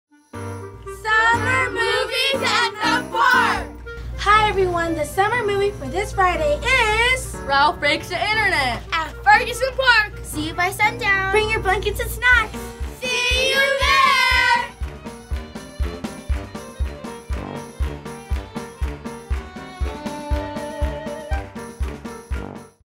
Summer Movie for June 14th
What's the next movie in the park for this Friday, June 14th? The Rialto Summer Camp Kids are here to let you know!